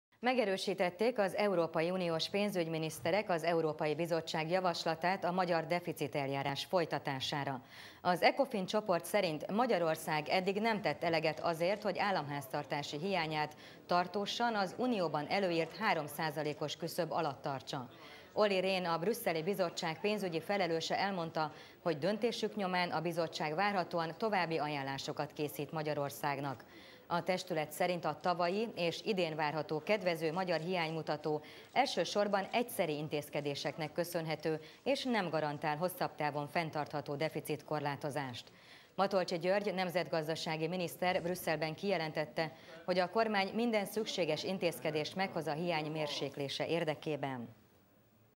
Deficiteljárás: az uniós pénzügyminiszterek is léptek a magyar költségvetés miatt
Megerősítették az Európai Unió tagországainak pénzügyminiszterei kedden az Európai Bizottságnak azt az értékelését, hogy Magyarország eddig nem hozott elegendő intézkedést annak érdekében, hogy államháztartási hiányát tartósan az unióban előírt 3 százalékos küszöb alatt tartsa.